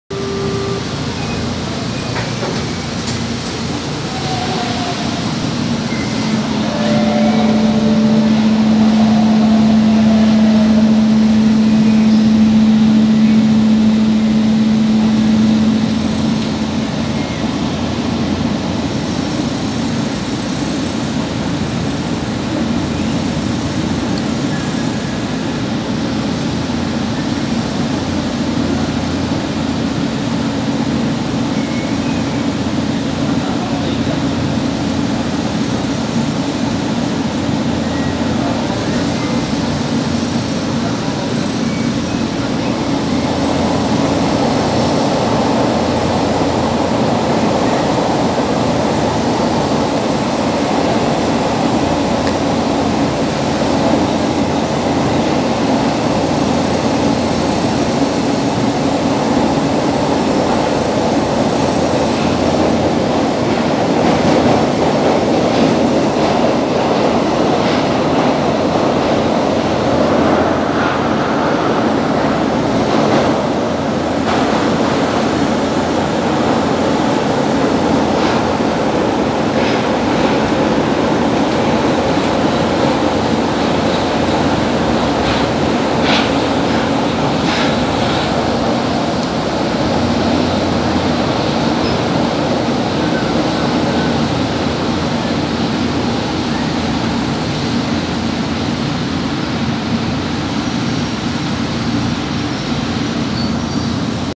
Trecho entre Parada Inglesa e Tucuruvi no TUE E02 - Linha 1 Azul
Filmado no E02 (carro E021)
Data: 17/12/18

Reparem que o chopper do E021 tá alto!